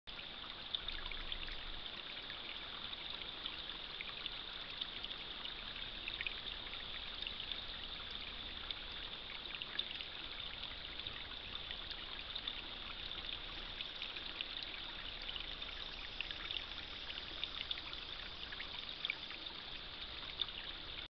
Eurasian Sparrowhawk (Accipiter nisus)
The Eurasian Sparrowhawk is eating its morning meal.